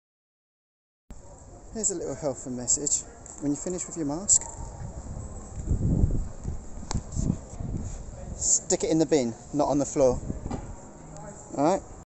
When Your Finished With Your Mask Stick It In The BIN Not On The Floor.
Getting Fed Up With Seeing Used Masks Laying On The Floor. When Your Finished With Them Stick It In The BIN.